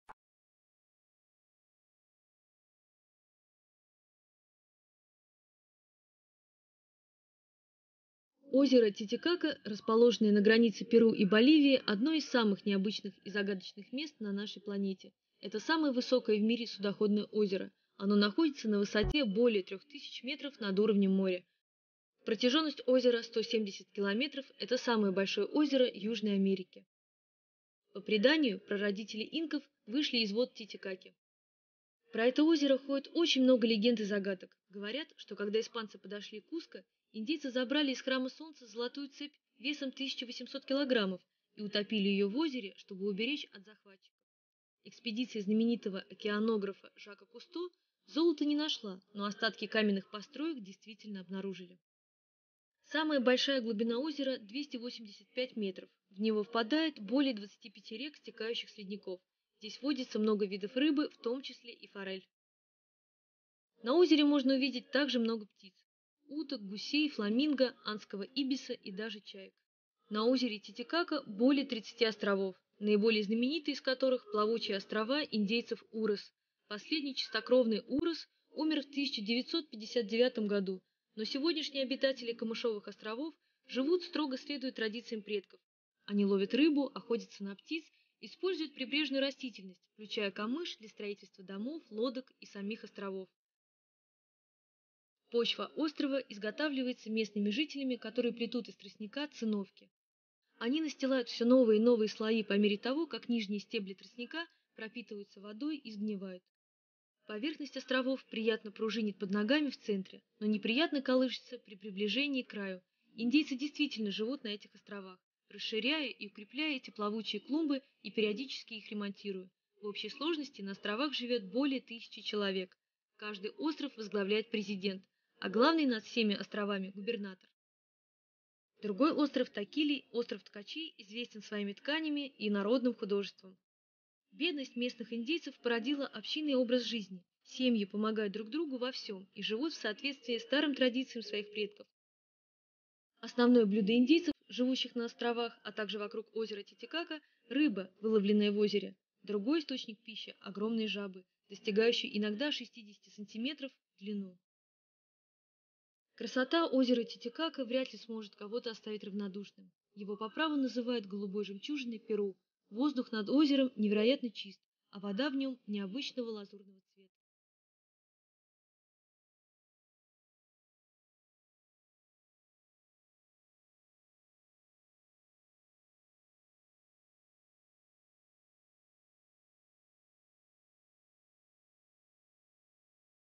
ОЗЕРО ТИТИКАКА
Экскурсия на лодке по озеру Титикака недалеко от Пуно, Перу Индейцы племени урос живут на плпвающих островах, связанных из тростника.